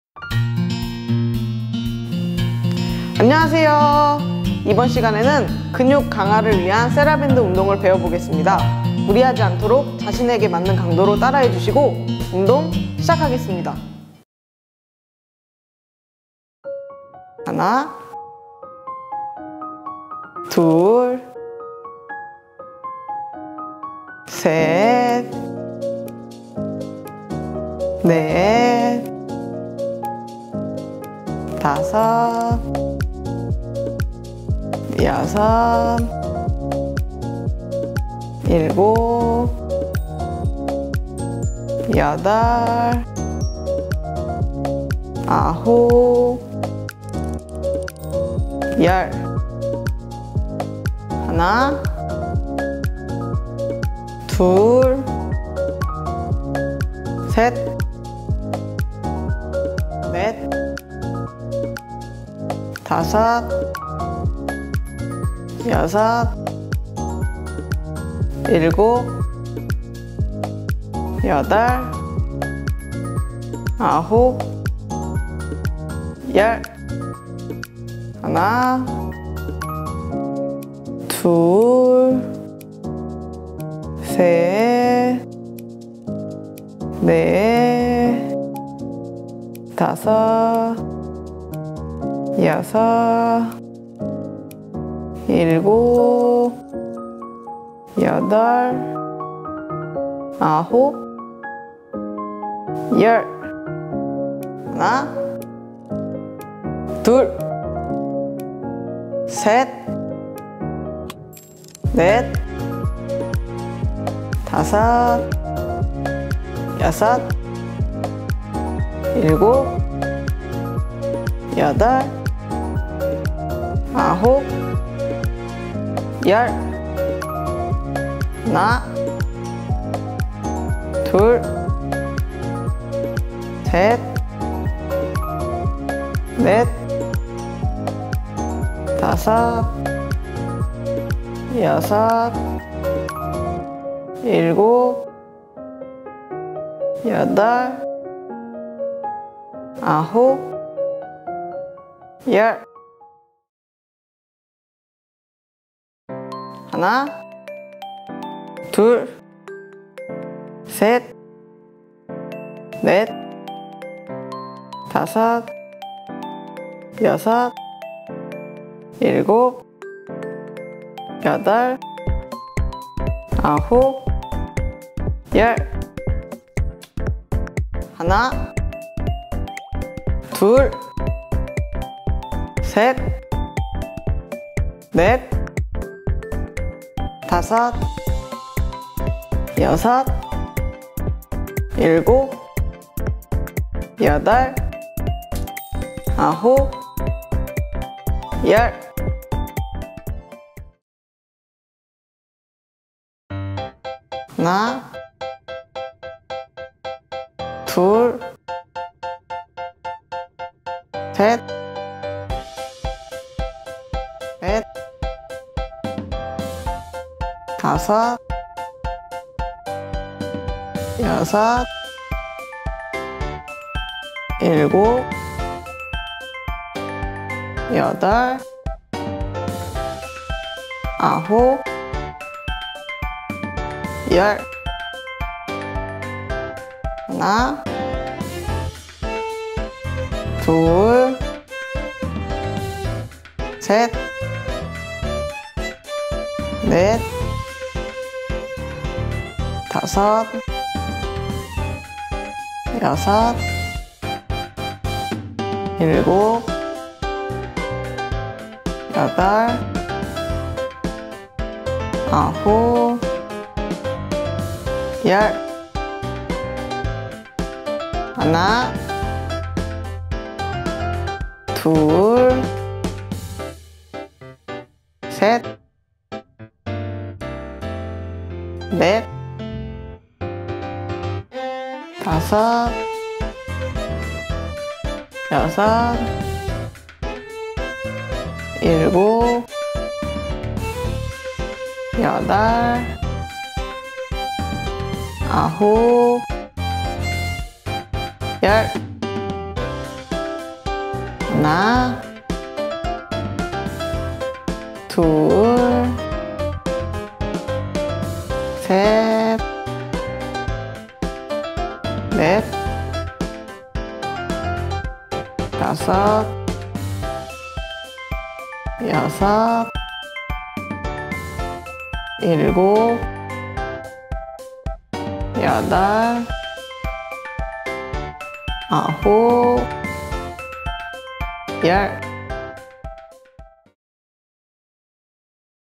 # 12. 세라밴드를 이용한 전신운동 1탄
세라밴드를 이용한 운동은 밴드의 저항으로 인한 운동으로 부상의 위험이 낮아 어르신들이 쉽게 따라 할 수 있는 운동입니다.

# 세라밴드를 이용한 전신운동
1. 어깨 돌리기
2. 팔 가슴 당기기
3. 양팔 기지개 펴기
4. 옆구리 당기기
5. 발끝 밀기
6. 양팔로 노젓기
7. 누워서 다리 밀기
8. 누워서 다리 내리기